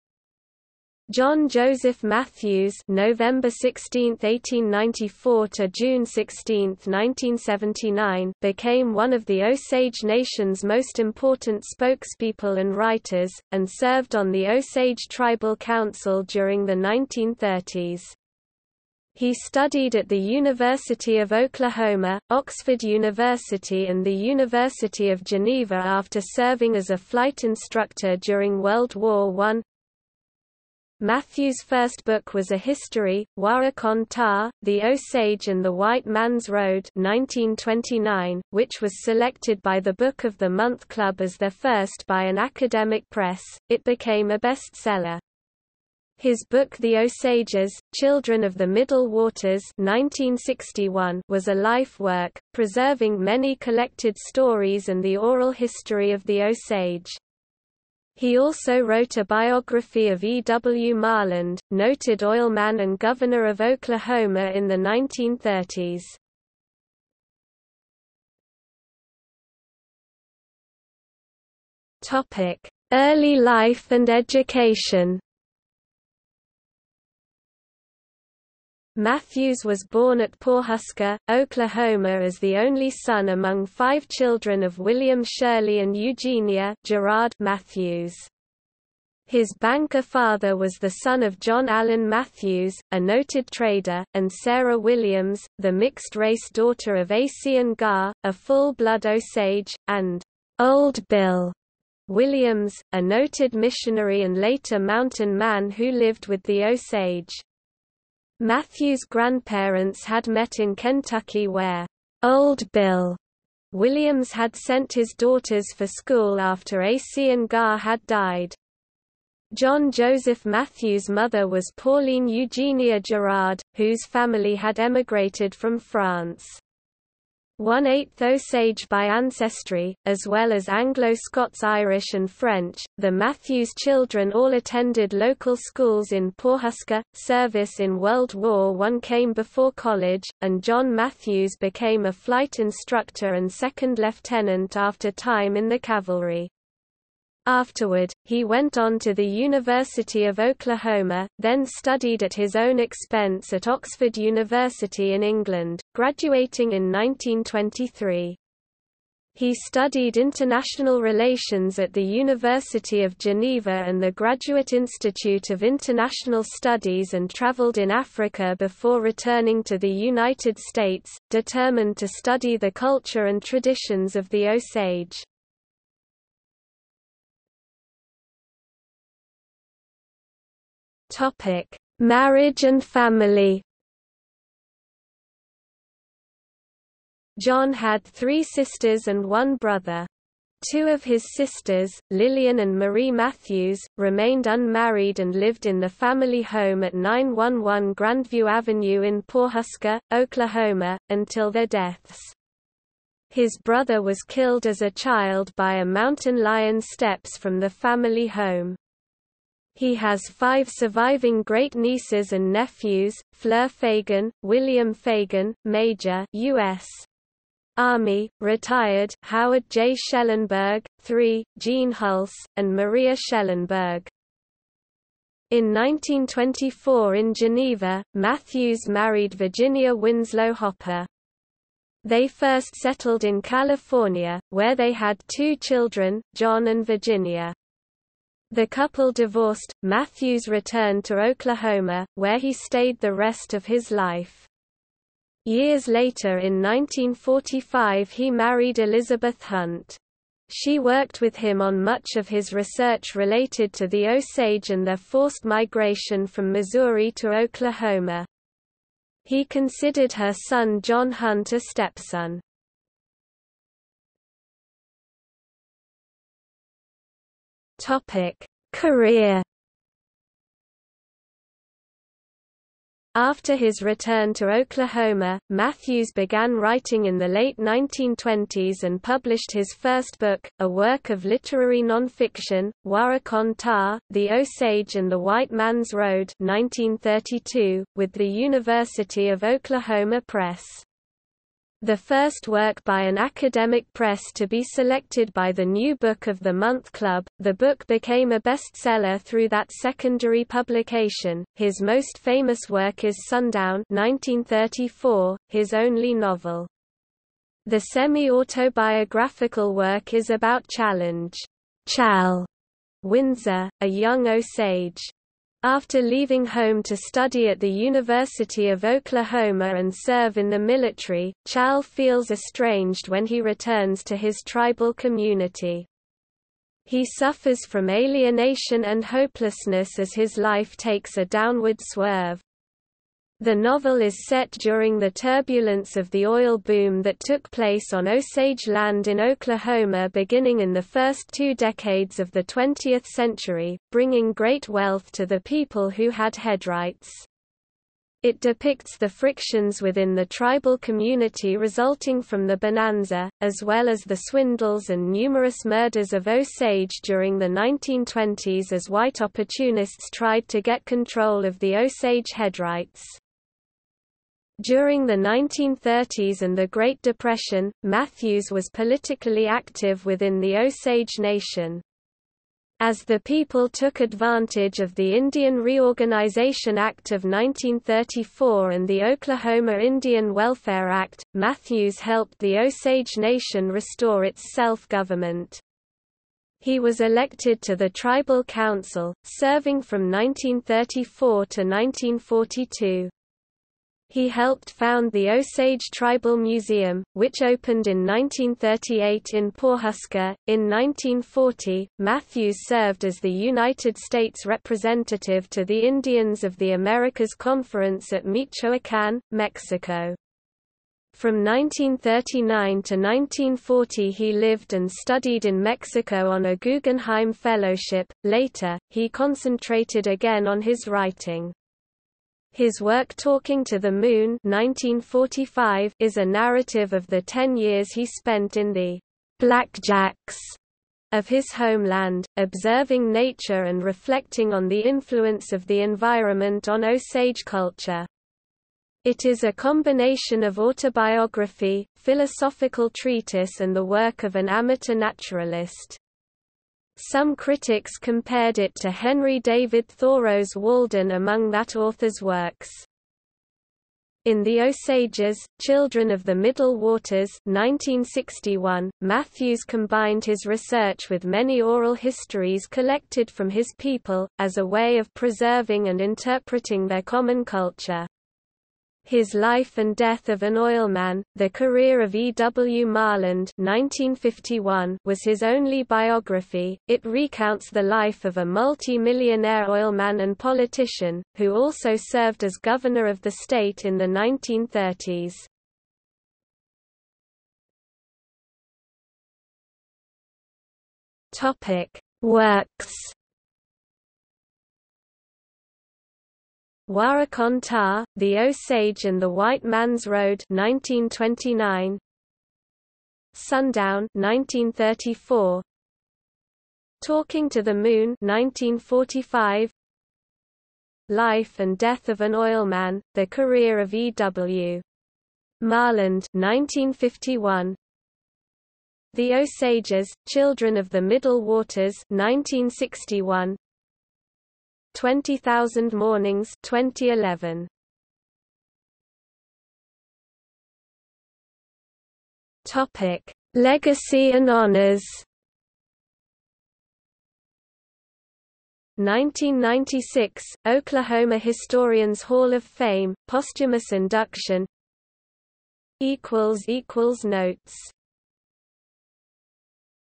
John Joseph Mathews | Wikipedia audio article
00:01:08 1 Early life and education
00:02:57 2 Marriage and family
00:04:24 3 Career
00:08:07 4 Works
00:08:49 5 Legacy and honors

- Socrates

SUMMARY
John Joseph Mathews (November 16, 1894 – June 16, 1979) became one of the Osage Nation's most important spokespeople and writers, and served on the Osage Tribal Council during the 1930s. He studied at the University of Oklahoma, Oxford University and the University of Geneva after serving as a flight instructor during World War I.
Mathews' first book was a history, Wah'kon-tah: The Osage and The White Man's Road (1929), which was selected by the Book-of-the-Month Club as their first by an academic press; it became a bestseller. His book The Osages: Children of the Middle Waters (1961) was a life work, preserving many collected stories and the oral history of the Osage. He also wrote a biography of E. W. Marland, noted oilman and governor of Oklahoma in the 1930s.